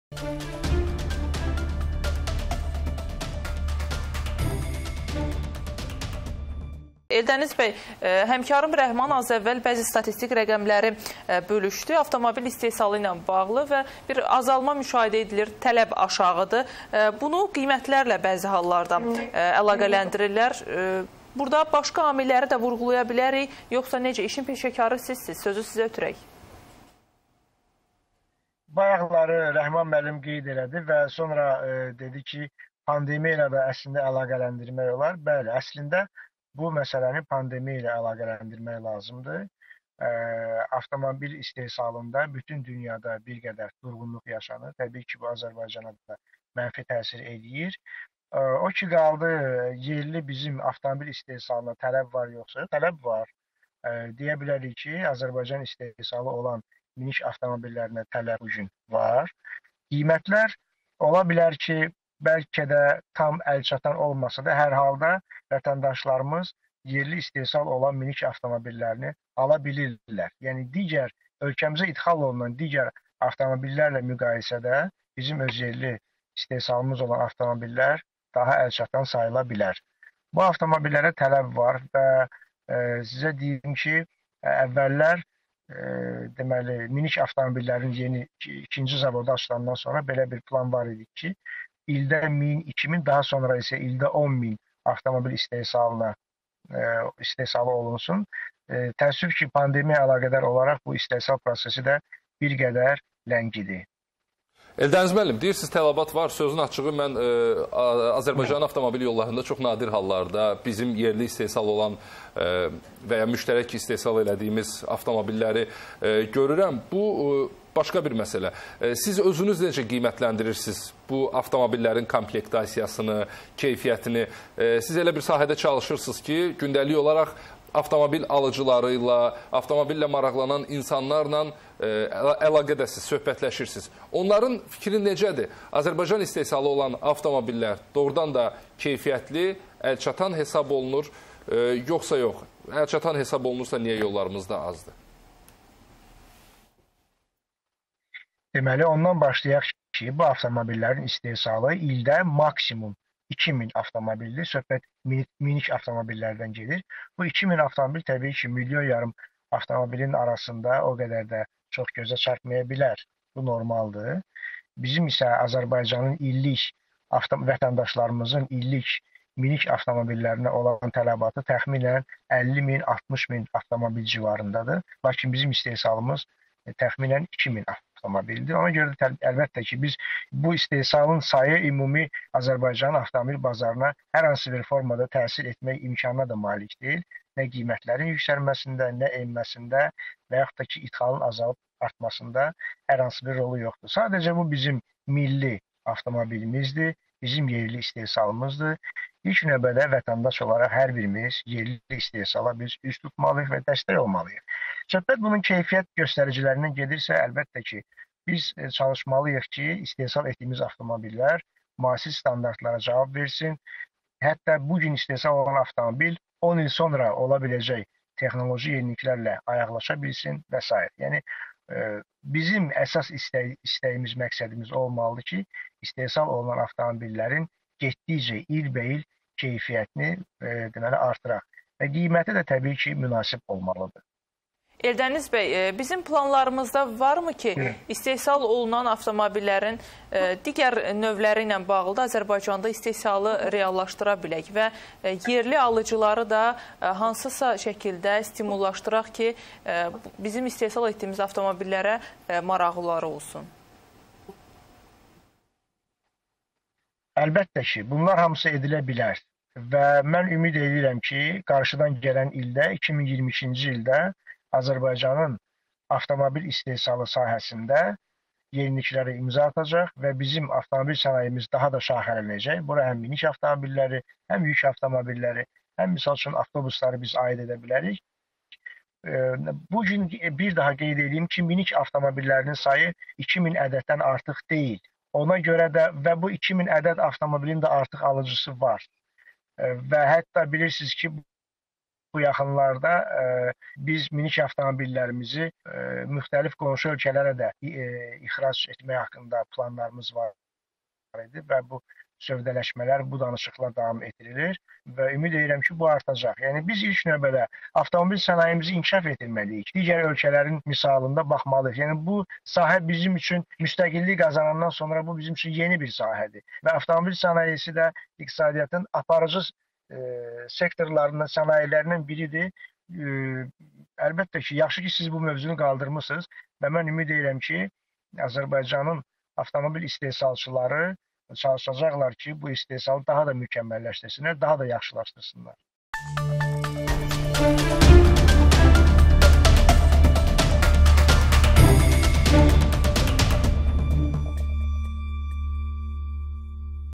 Ölkədə istehsal edilən avtomobillər bazara hər hansı formada təsir edə bilməz - İş vaxtı
Qonaq: Eldəniz Cəfərov